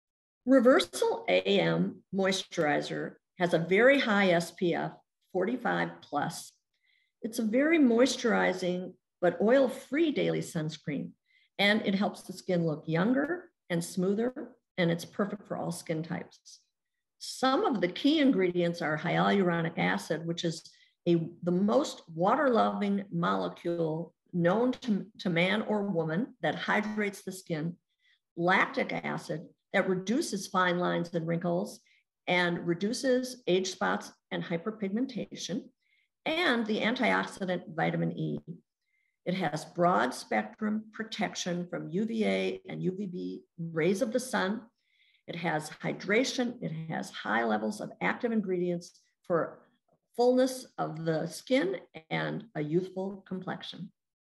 AM Daily Moisturizer SPF 45+
AM Daily Moisturizer SPF 45+ is an oil-free, extremely moisturizing sunscreen and anti-aging complex formulated with hyaluronic and lactic acids and vitamins E and B5. It provides broad-spectrum protection while making skin appear younger and smoother.